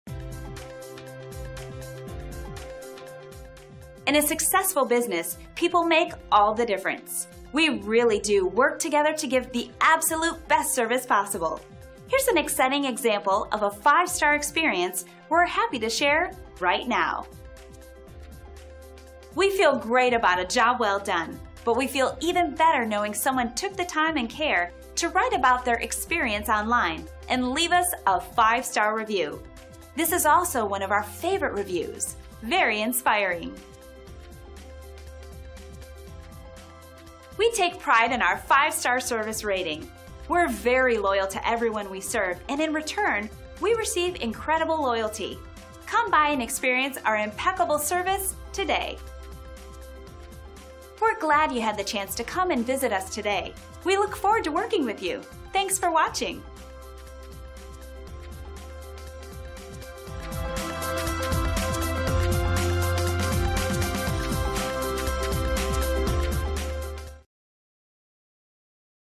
Seibro, Inc. Guntersville Terrific 5 Star Review by Larry R.
New Rating

        Don Brown is the consummate Pro! His web design capabilities are superior.He makes your company look really great using eye-popping videos. Also, the unique Review Process that Seibroinc employs, easily helps you build your reputation. Seibroinc can provide most any aspect you or your company needs to maximize your web presence.

        Seibro, Inc.
       241 Bennett Dr.
        Guntersville AL
        35976